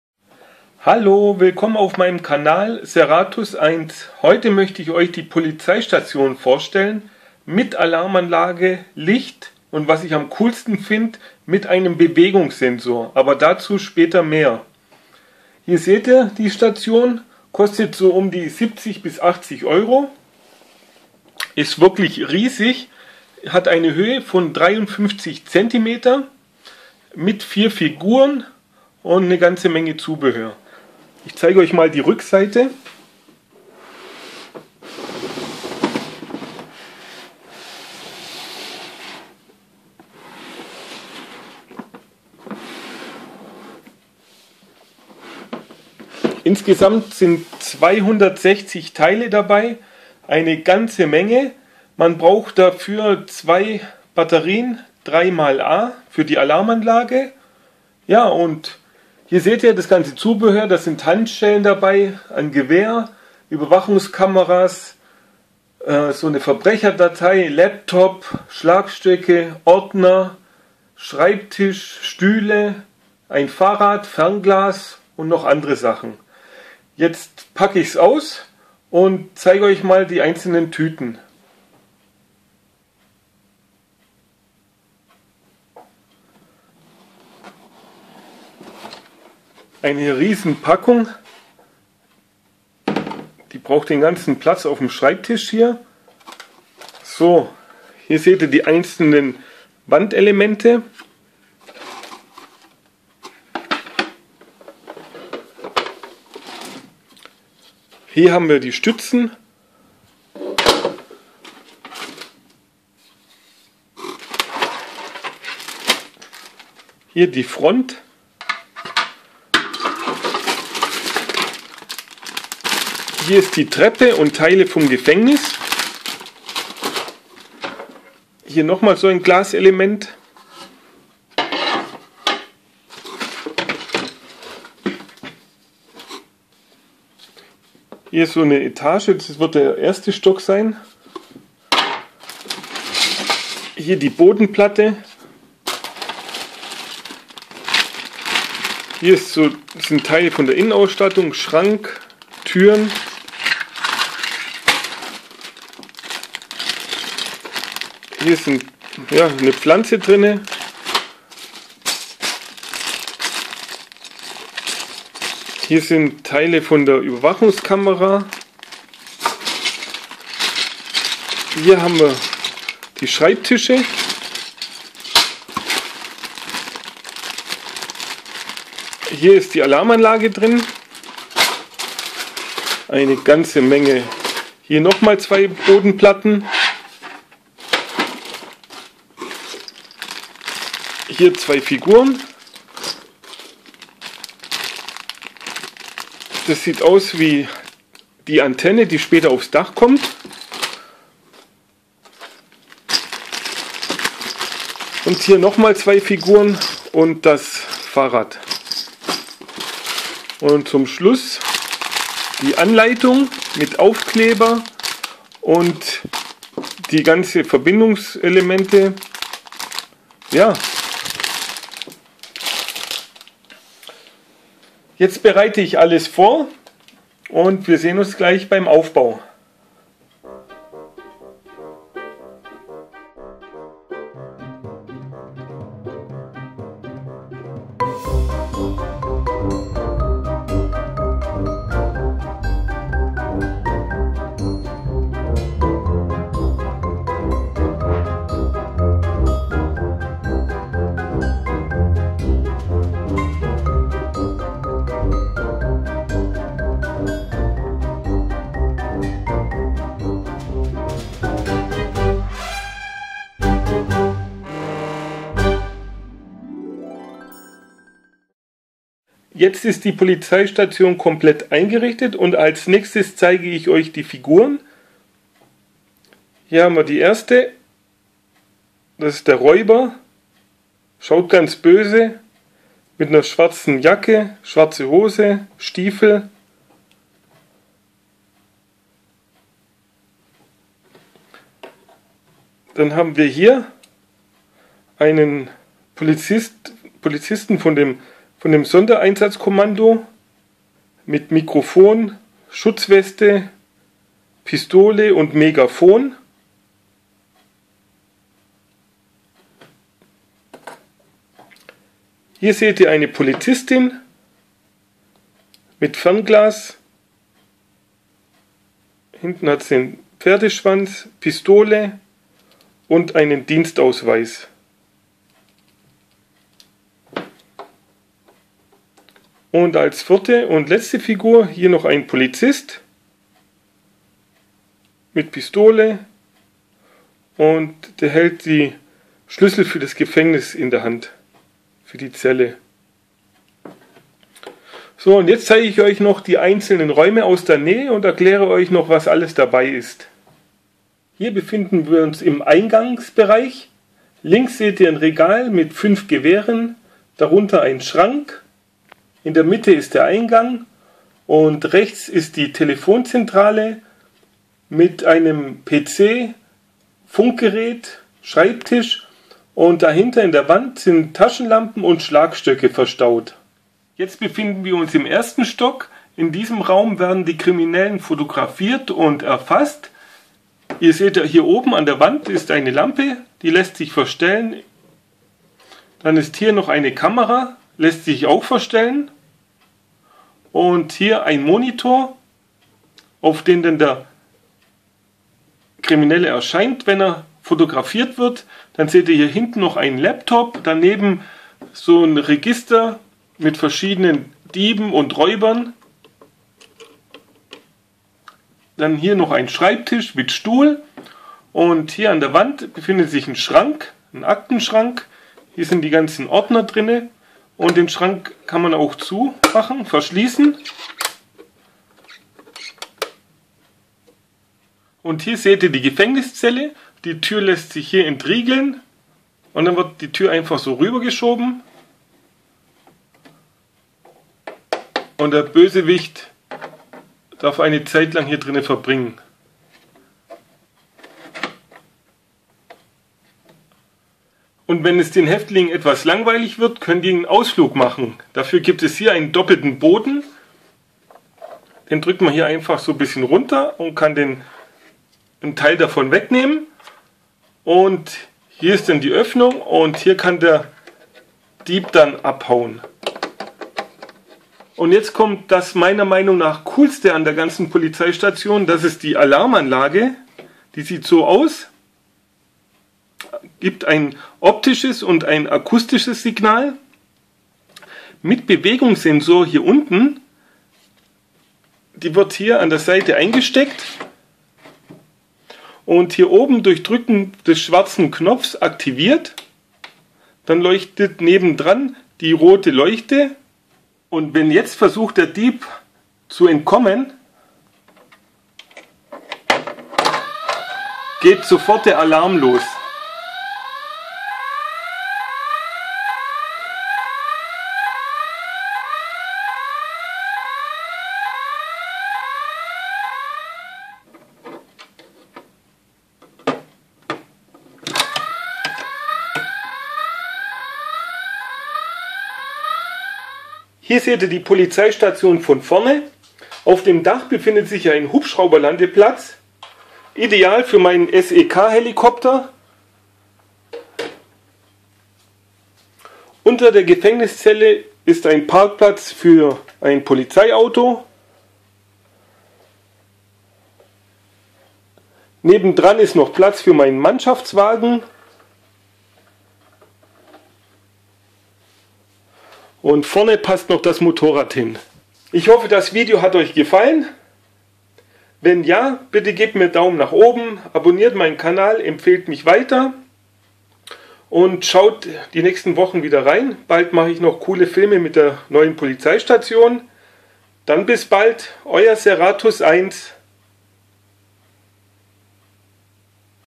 Playmobil Polizeistation 5176 auspacken seratus1 Alarm Policestation
Auspacken und Aufbau der riesigen Polizeistation. Mit Alarmanlage und Bewegungssenor. Ich zeige euch die Einrichtung und Funktionen.

Playmobil Polizeistation bei amazon

Lade dir kostenlos die seratus1 App runter:
seratus1.zur.app